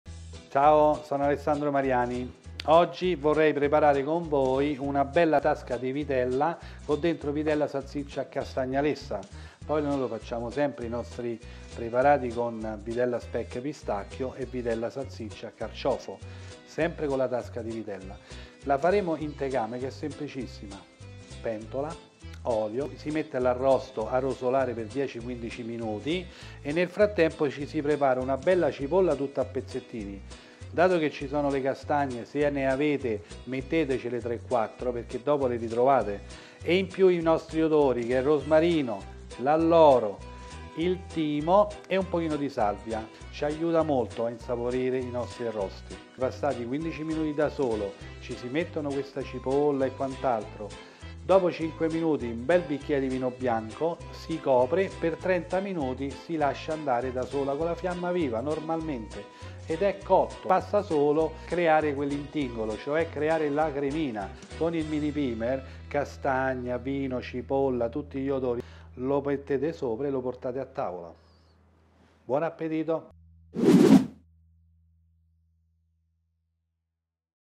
Come preparare la tasca di vitella con salsiccia e castagne lessa
Una magnifica tasca di vitella che contiene altra vitella, salsiccia e castagne lesse. Una delizia che trovate tra i nostri preparati in macelleria e che scopriamo come cucinare seguendo le istruzioni di Alessandro Mariani